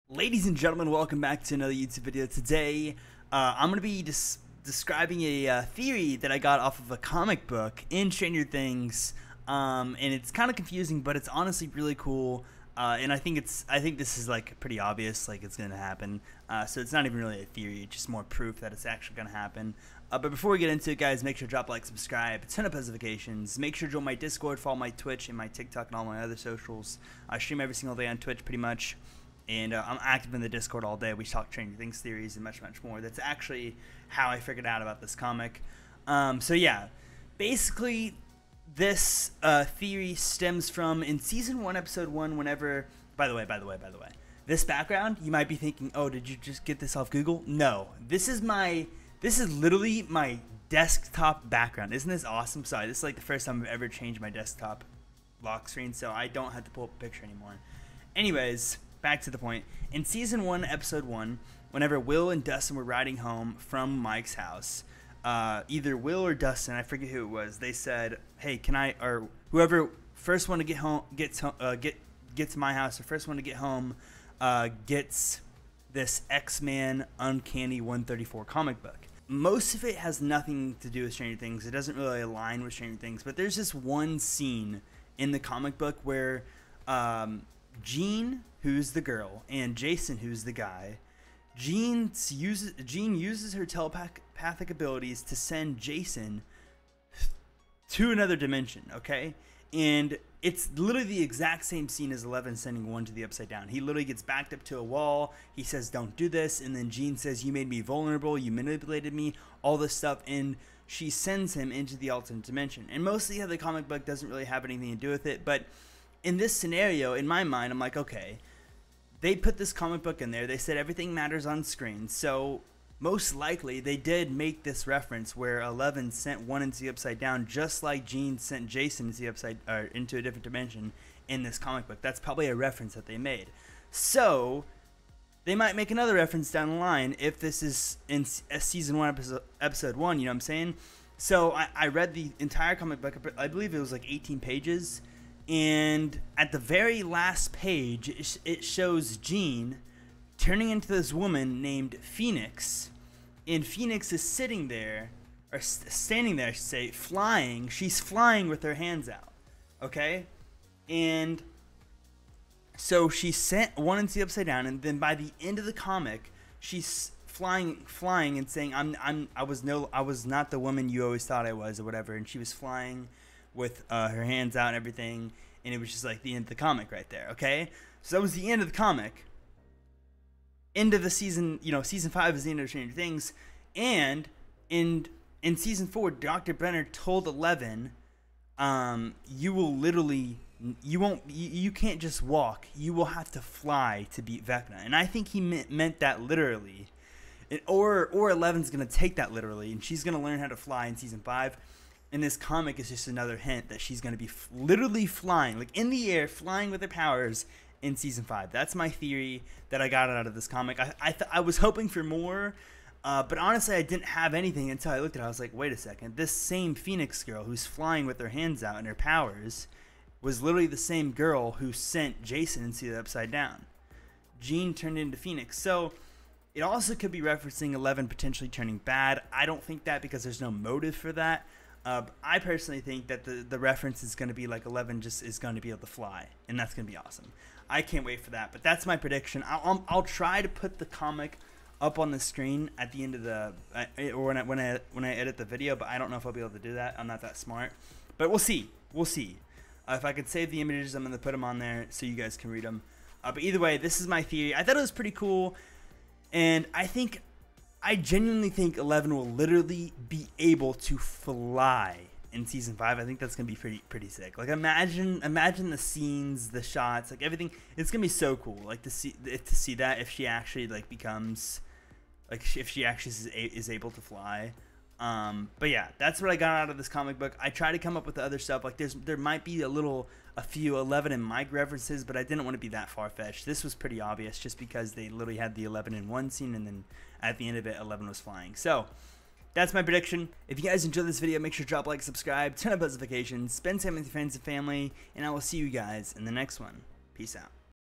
Eleven will be able to FLY in Season 5 (THEORY)
Eleven will literally be able to FLY in Season 5

The Entire Stranger Things Storyline Explained!